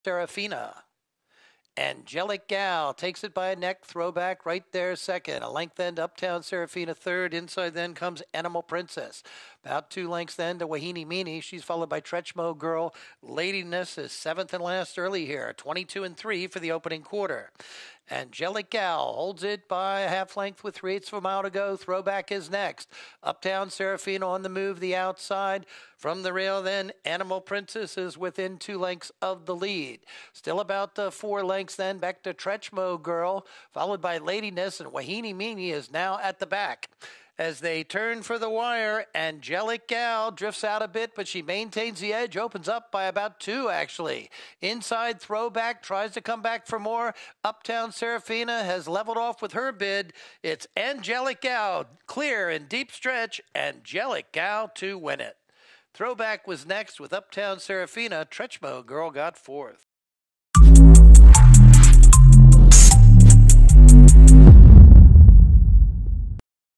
5ª. CARRERA - ANGELIC GAL con Wilfred Vásquez
SE CRECIÓ
Así ganó "El Gato" Wilfred Vásquez la 5ª. Carrera de hoy jueves 21/12 en Penn National con el ejemplar #2 ANGELIC GAL en punta y se creció

Lote: 1/2 Furlongs | Fillies and Mares | 3 Year Olds And Up | CLAIMING : $5,000 | PURSE: $11,156

Pizarra:
2Angelic GalJ: W. Vasquez 121Lbs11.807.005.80
7ThrowbackJ: I. Beato 121Lbs 4.603.2
9Uptown SeraphinaJ: D. Cora 121Lbs  3.80

Times in 5ths: :223   :462   :583   1:05